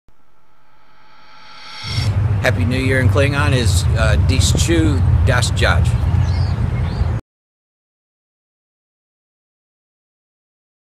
New Year's Greetings In 2 Languages
Ex-Indian Trail Councilman David Waddell Offers Greetings In English, and Klingon.